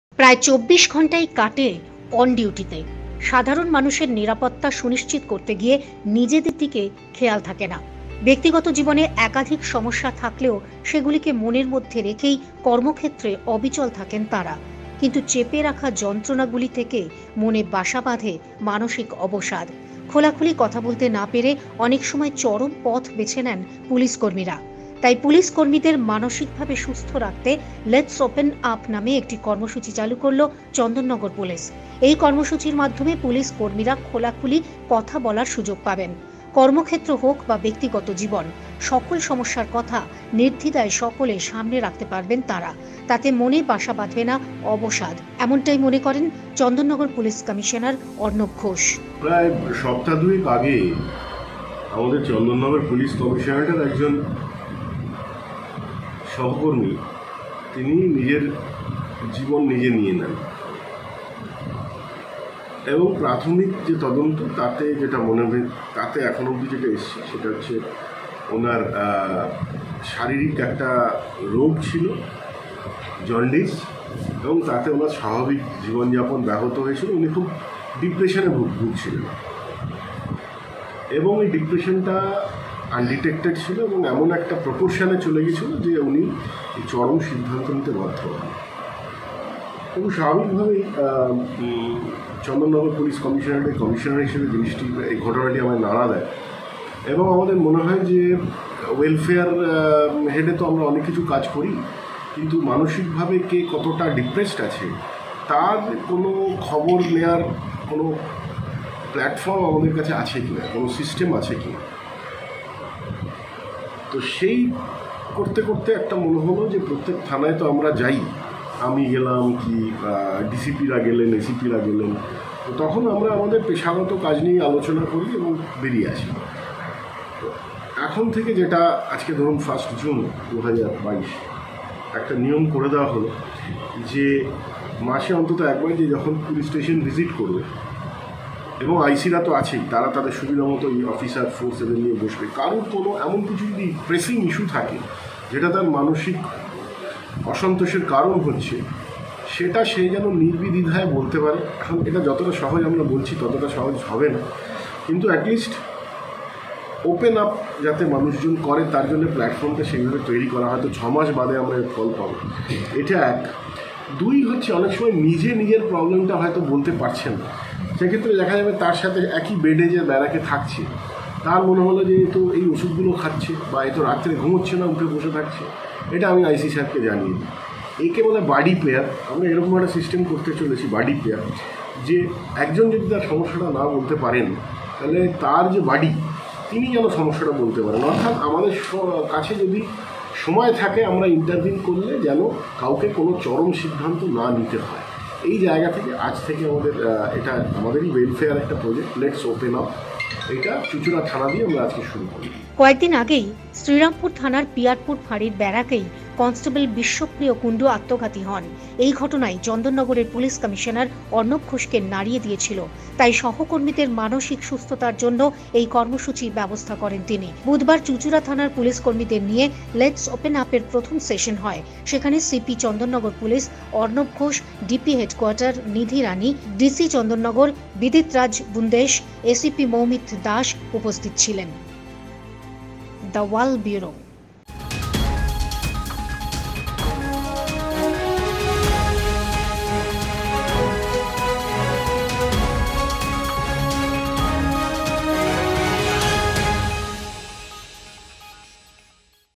অবসাদ নিরাময় করতে খোলাখুলি কথা বলবেন পুলিশকর্মীরা
⭐ Pujo Porikroma

⭐ Aparajeyo

⭐Pujo Parba